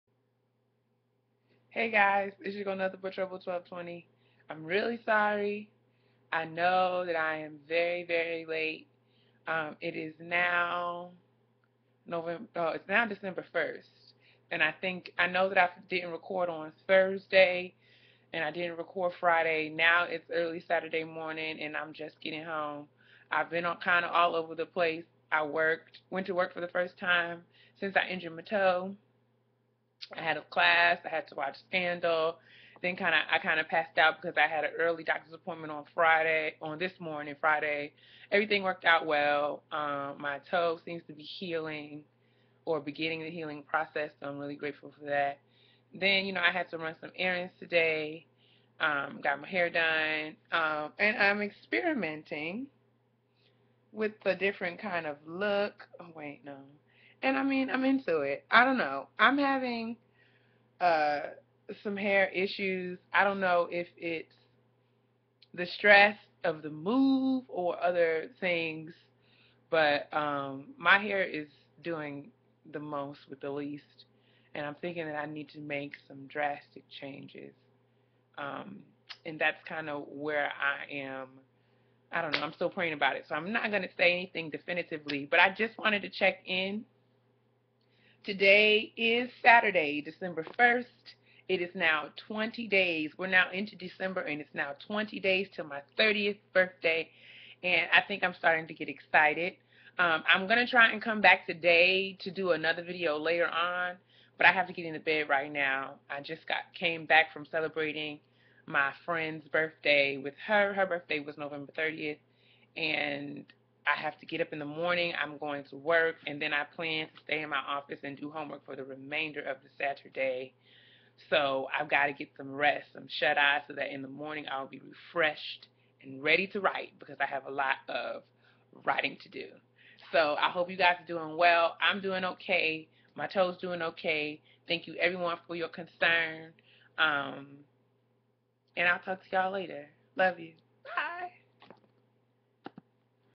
30 in 30 Days - Days 10, 11 and a lil bit of Day 12
I know I'm late but it's been a hectic week... finals and things of that nature. i'll make it up with you with two videos today... or at least that's the plan.